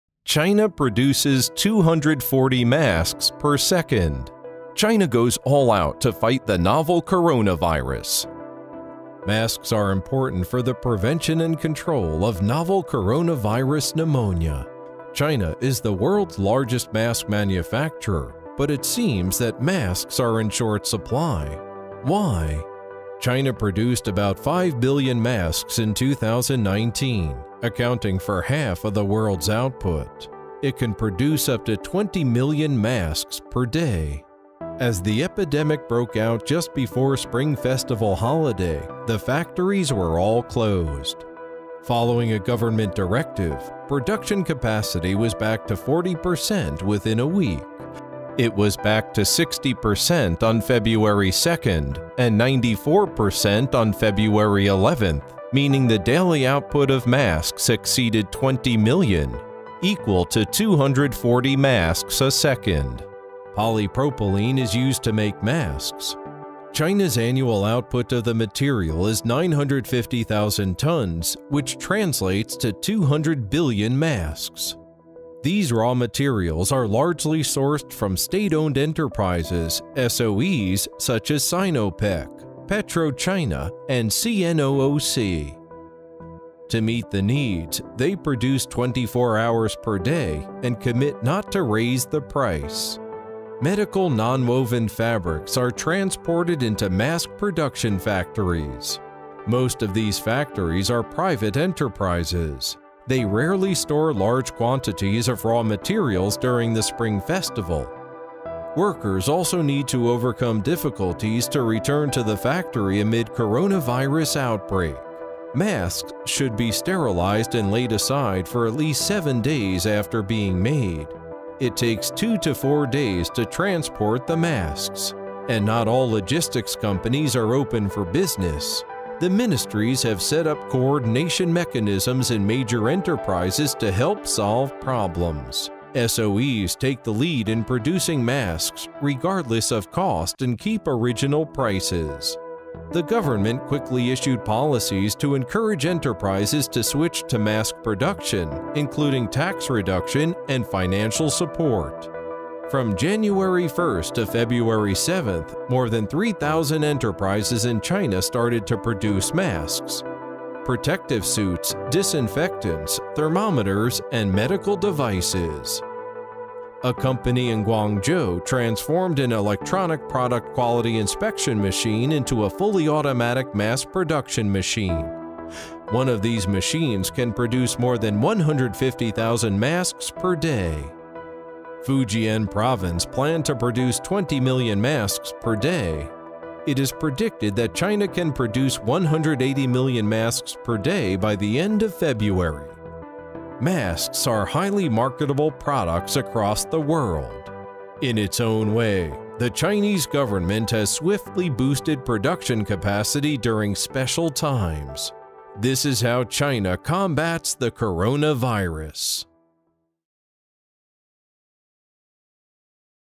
How does China combat coronavirus: 240 masks produced per second!
China can produce up to 240 masks per second. But since the novel coronavirus broke out in Wuhan, it seems that masks are in short supply. Why?
And how does China swiftly boost medical product production to combat the coronavirus outbreak? Find out: xhne.ws/i0JYm